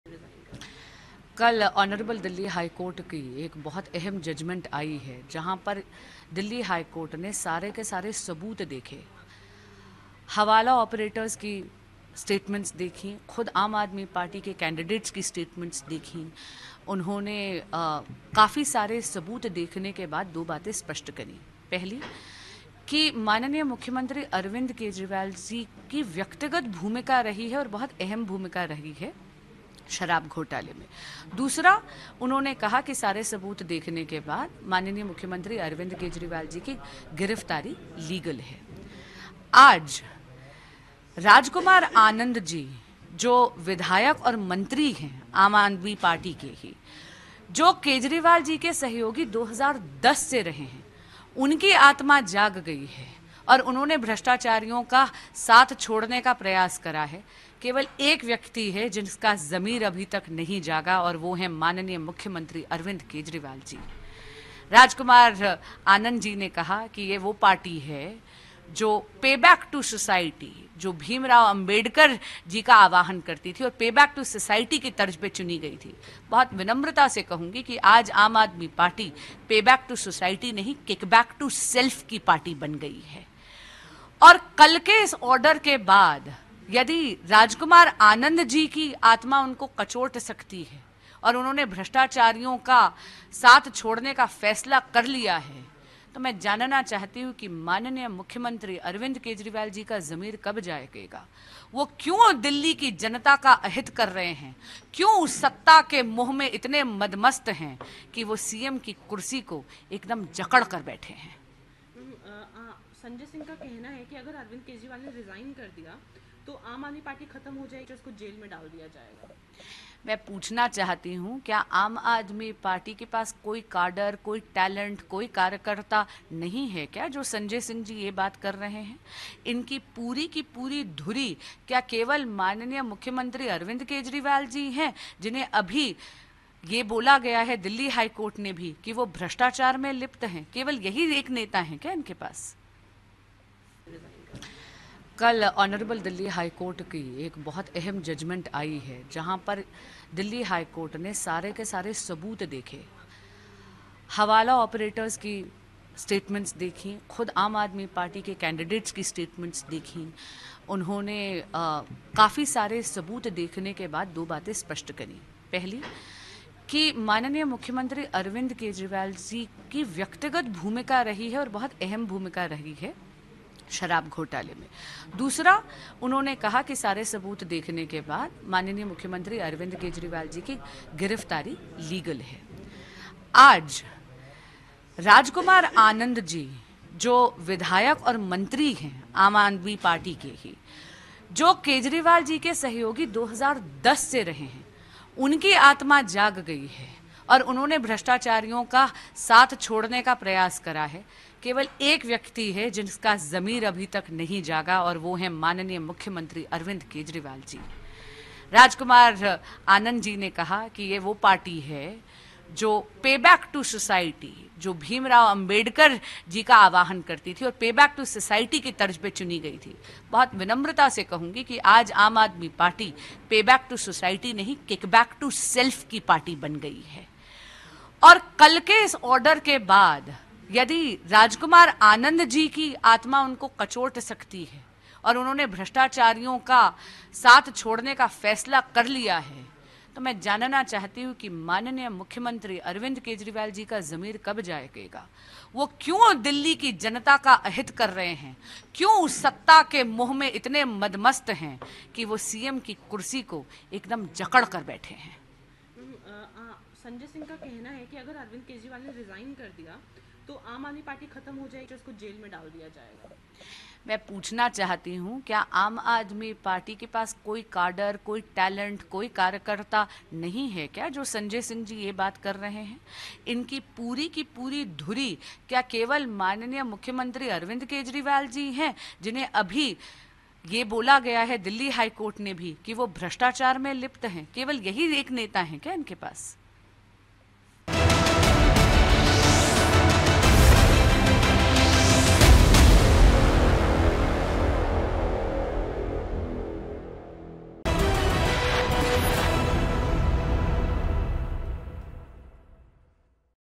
Bansuri Swaraj: Court ने कहा Arvind Kejriwal की गिरफ्तारी सही है Bansuri Swaraj | Loksabha Election
BJP प्रत्याशी Bansuri Swaraj केजरीवाल पर  हमला बोला है । उन्होंने कहा कि कोर्ट ने कहा है कि 'शराब घोटाले में केजरीवाल की व्यक्तिगत भूमिका रही है

BJP candidate Bansuri Swaraj has attacked Kejriwal. He said that the court has said that 'Kejriwal has a personal role in the liquor scam.